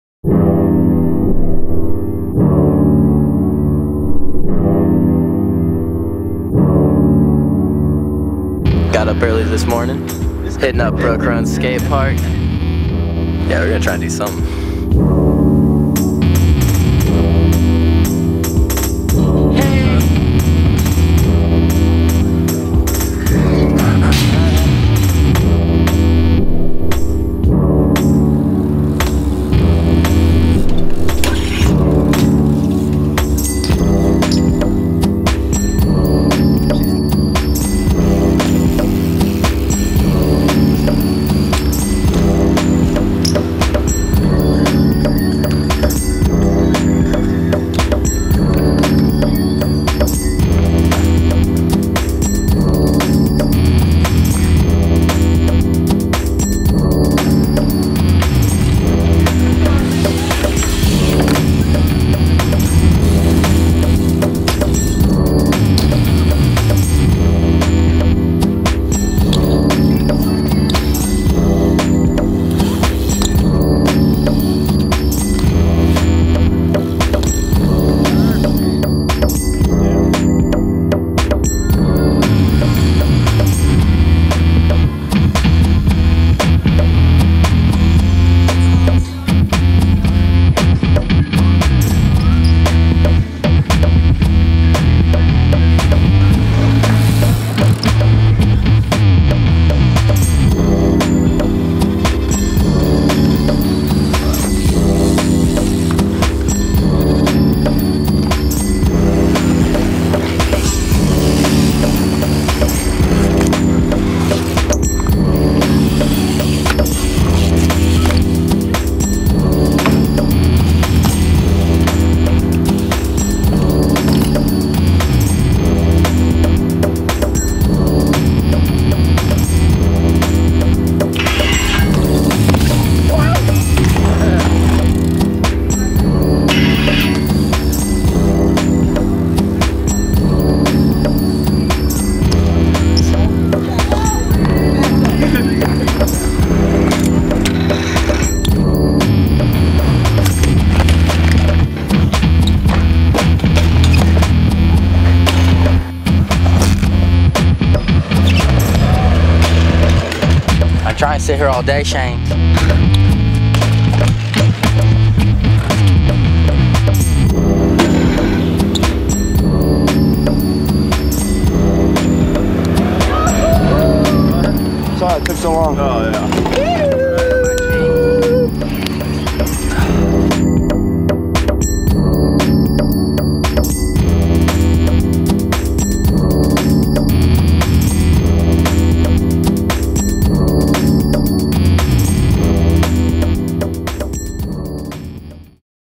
BMX - Stevie Churchill & Devon Smillie @ Brooke Run Skatepark
Stevie Churchill, Devon Smillie, Morgan Long and Shane Weston tore up the Brooke Run skatepark in Atlanta